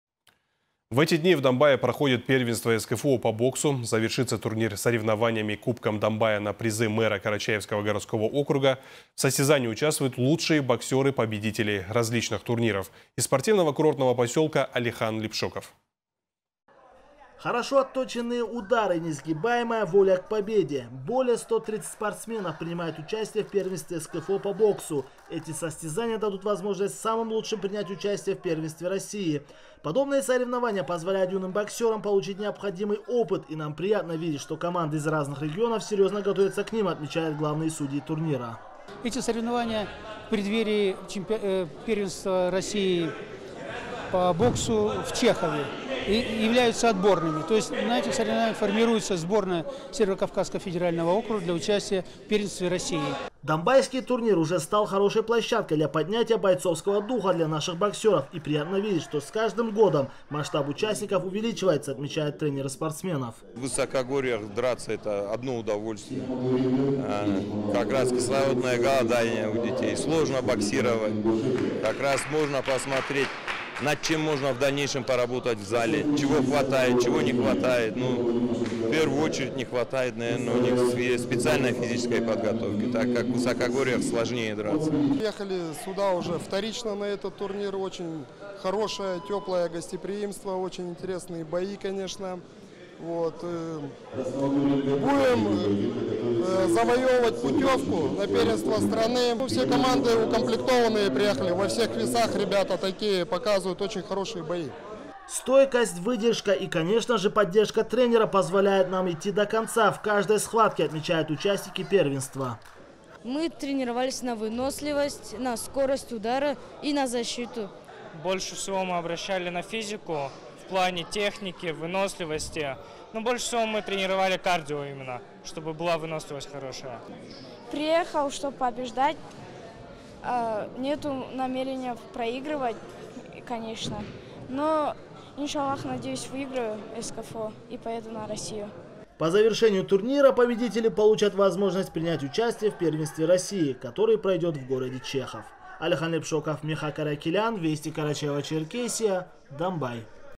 В эти дни в поселке Домбай проходит первенство СКФО по боксу
В эти дни в Домбае проходит первенство СКФО по боксу. Завершится турнир Кубком Домбая на призы мэра Карачаевского городского округа. В состязании участвуют лучшие боксеры, победители различных турниров.